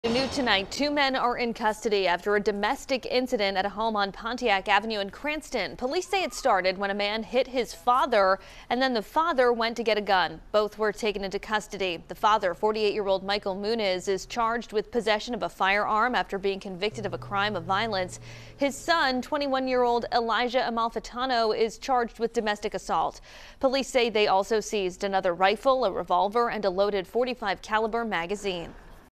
Police: Father, son in custody after domestic incident in Cranston
Cranston police say the father allegedly went to get an illegal firearm after his son allegedly hit him.